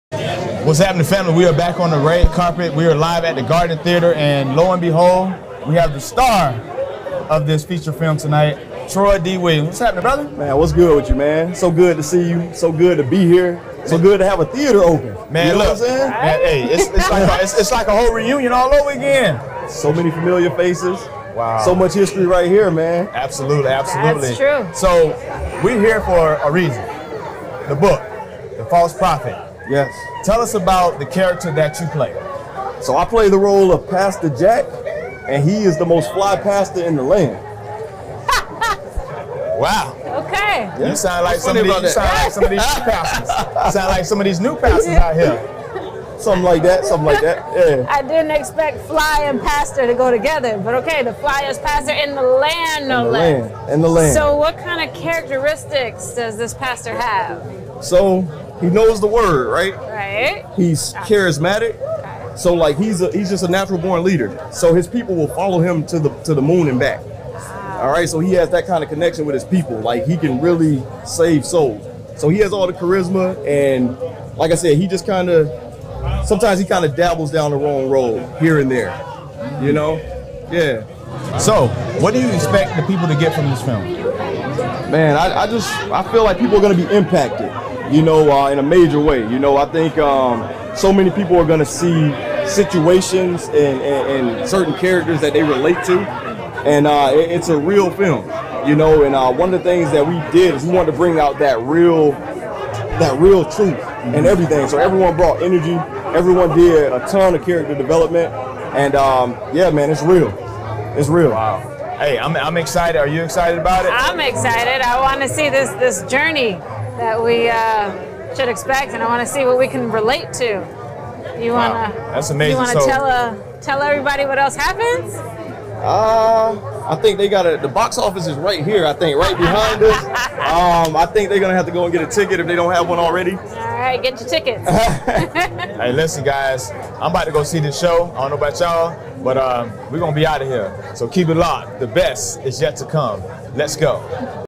Exclusive Red Carpet Interview
Exclusive red carpet interview at the private screening of The Book "False Prophet" with hosts TJ Chatman (The TJ Chatman Show) and Samantha Nicole.

Interview Filmed by Leslie Gray Photography